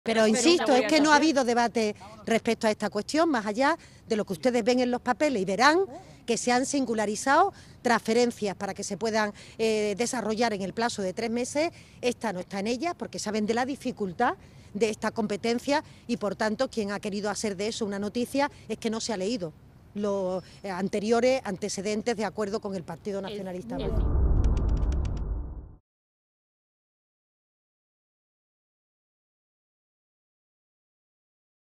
Montero, después del acuerdo PSOE-PNV: es "difícil" traspasar al País Vasco la Seguridad Social
Apenas unas horas después de que el PSOE y el PNV hayan firmado el acuerdo de investidura de Pedro Sánchez, y en el que se incluye el traspaso de la Seguridad Social al País Vasco, María Jesús Montero ha matizado de manera sensible este punto. La ministra de Hacienda y número 2 socialista ha reconocido que este traspaso es "difícil". "No se ha singularizado nada respecto a la Seguridad Social", ha incidido de hecho Montero, restando así relevancia a este punto que se incluye en el acuerdo pero al que, en realidad, no se alude de manera directa.

Montero, horas después del acuerdo PSOE-PNV: es "difícil" traspasar al País Vasco la Seguridad Social